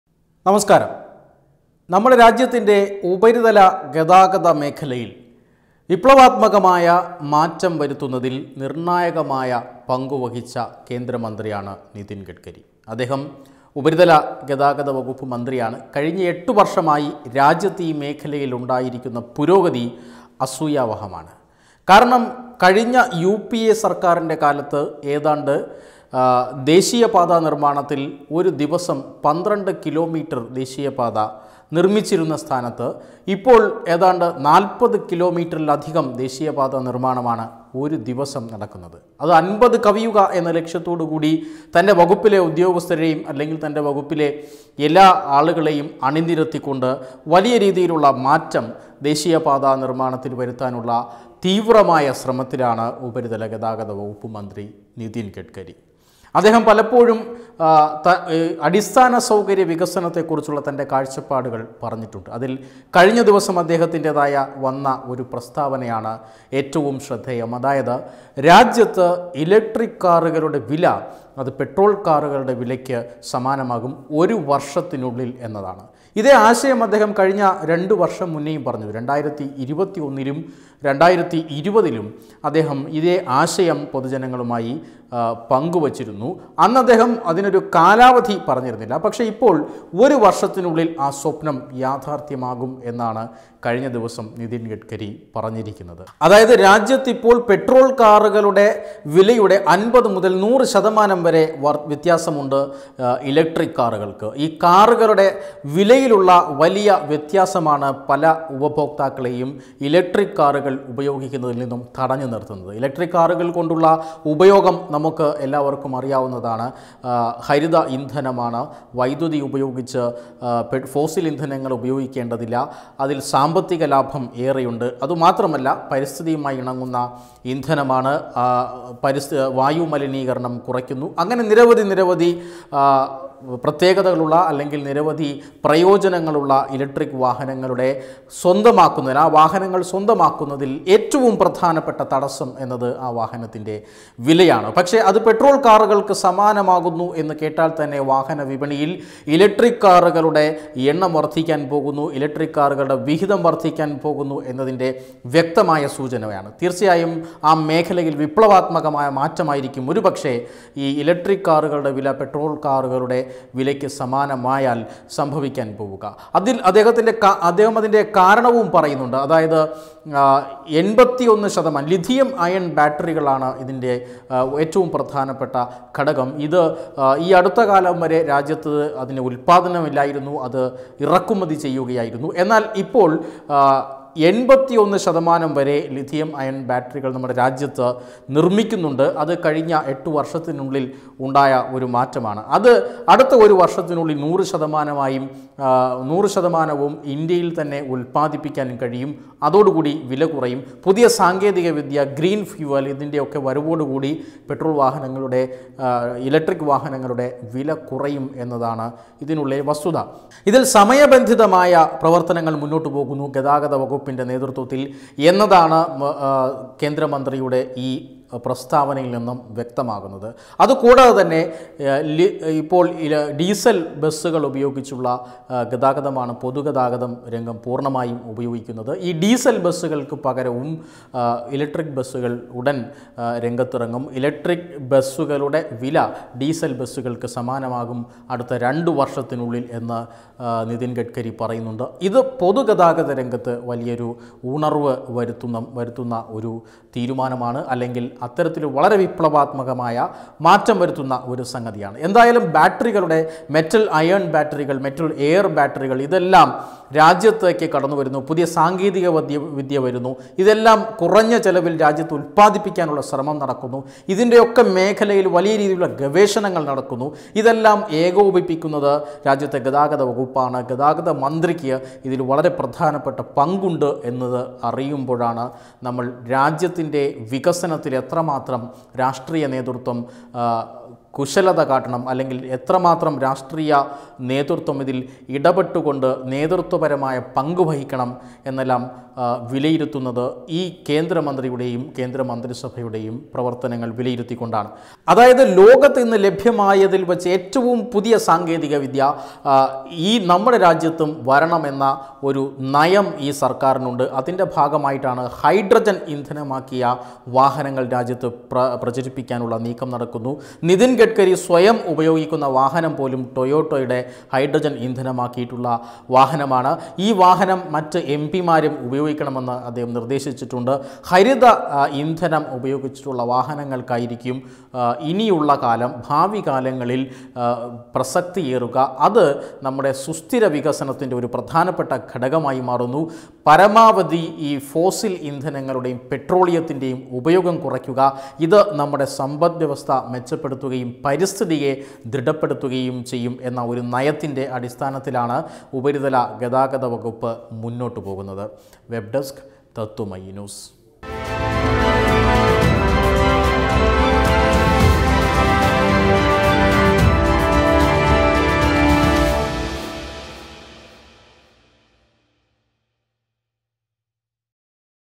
ലിഥിയം അയൺ ബാറ്ററികളുടെ ഉൽപ്പാദനം രാജ്യത്ത് 100 ശതമാനമായി ഉയരും
രാജ്യത്ത് ഹരിത ഗതാഗതത്തിന് ത്വരിത വേഗം വമ്പൻ പ്രഖ്യാപനങ്ങളുമായി നിതിൻ ഗഡ്‌കരി